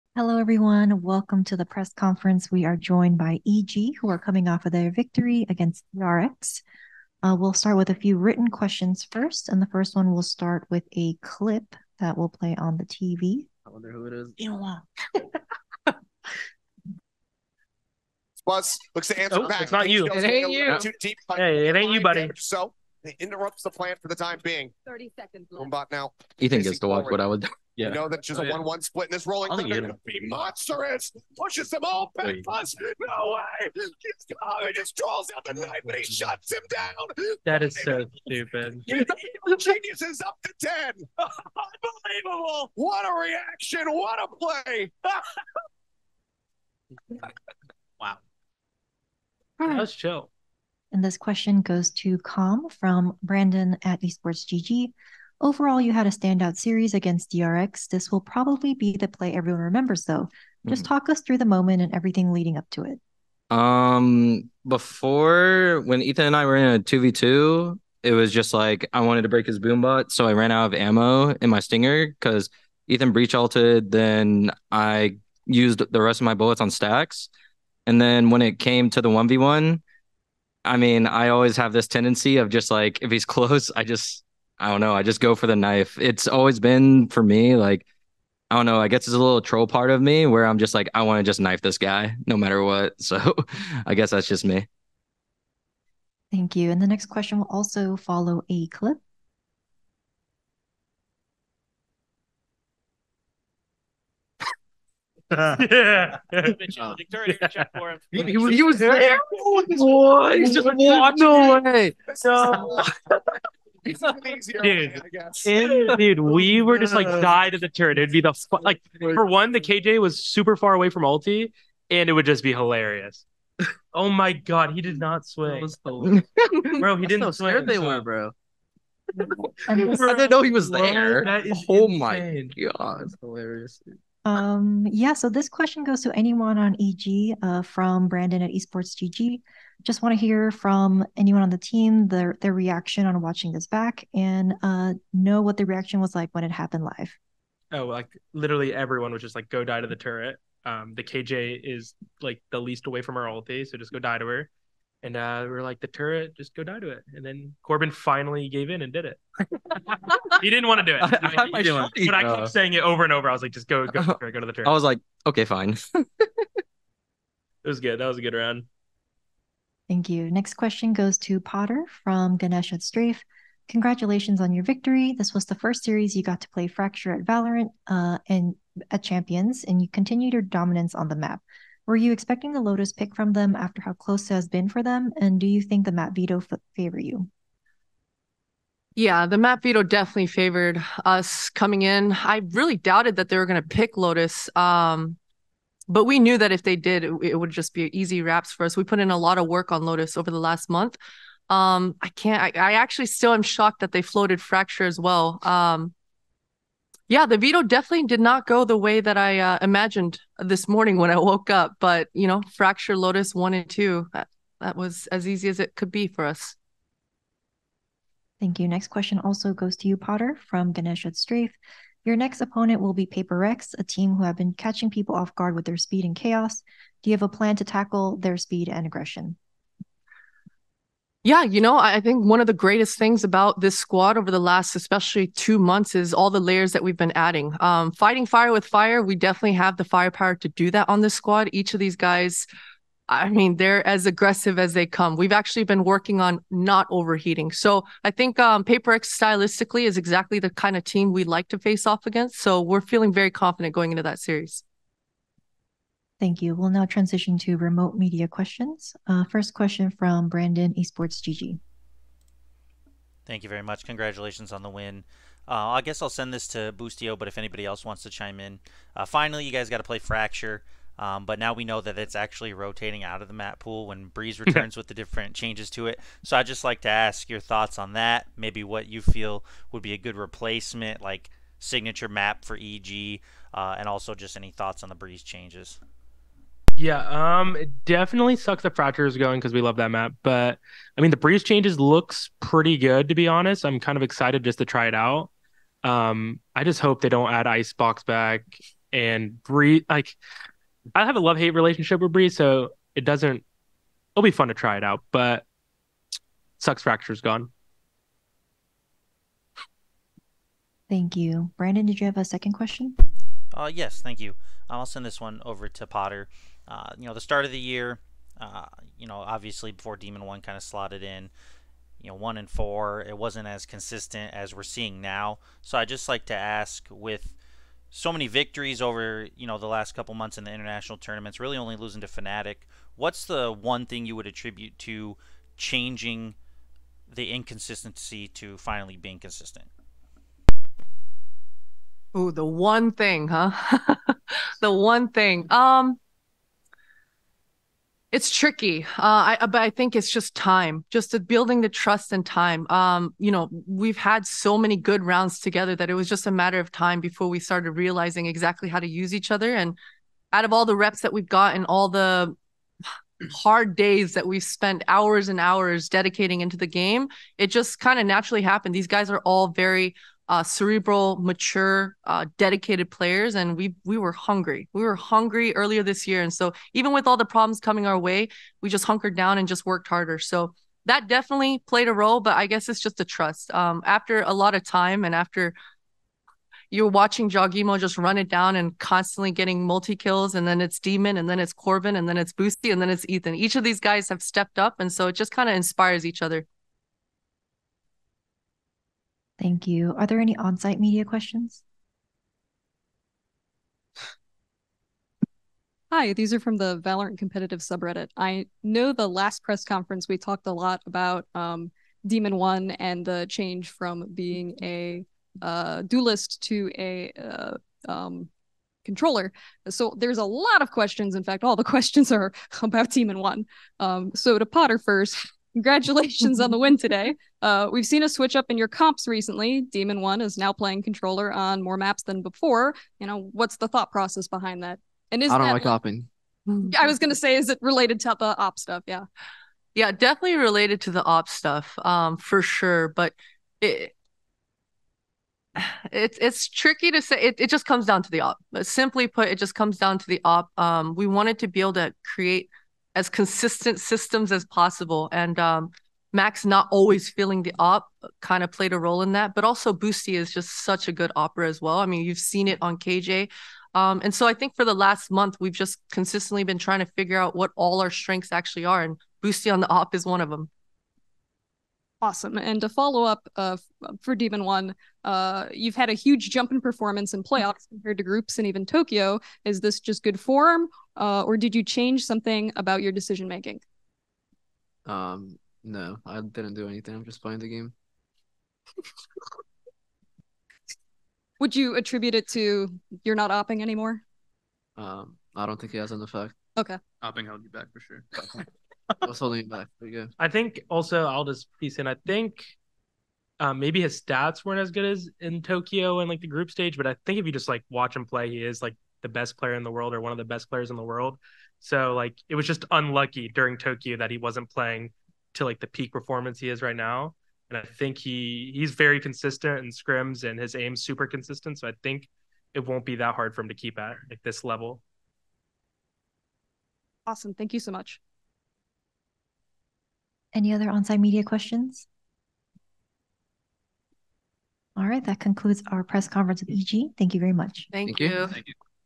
"I Just Want To Knife" EG Press Conference (EG vs DRX) VCT Champions Upper Bracket Semifinals
Evil Geniuses speak at their press conference after their win against DRX at VCT Champions 2023 in the upper bracket semifinals.

eg post game interview
eg post match interview